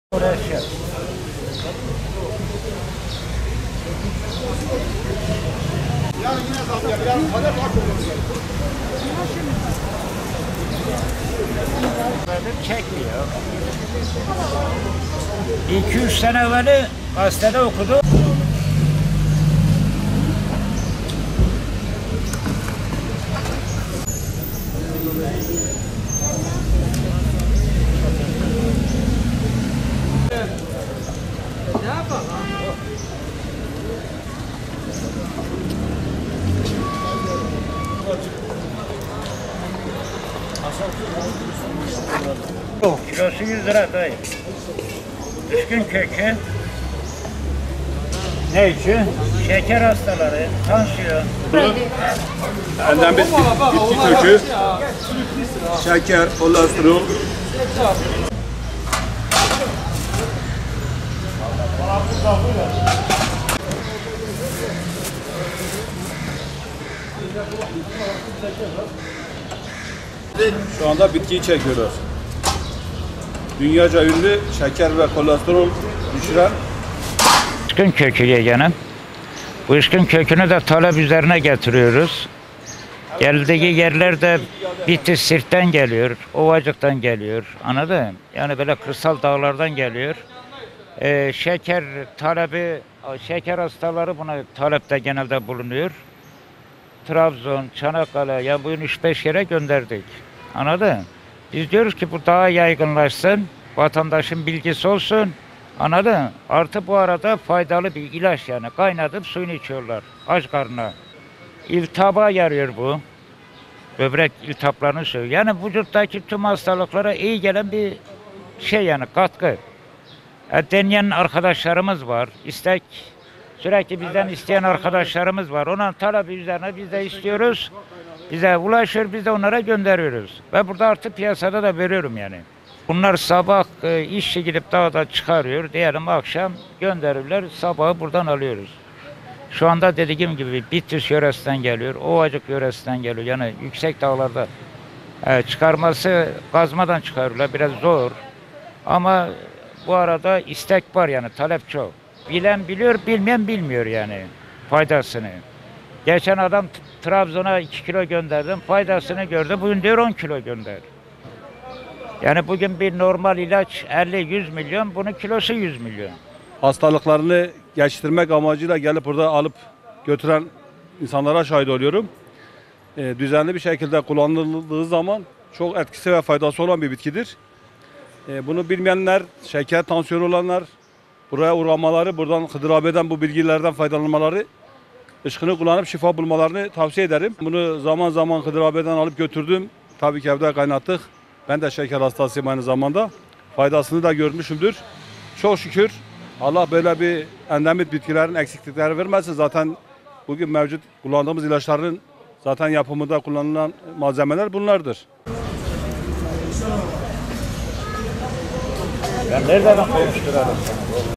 Şifa deposu: Işkın otu
Tunceli ve Bitlis’in yüksek kesimlerinde kazma yardımıyla toprak altından çıkarılan ve şifa deposu olarak bilinen ışkın otu kökü Elazığ’da tezgahtaki yerini aldı.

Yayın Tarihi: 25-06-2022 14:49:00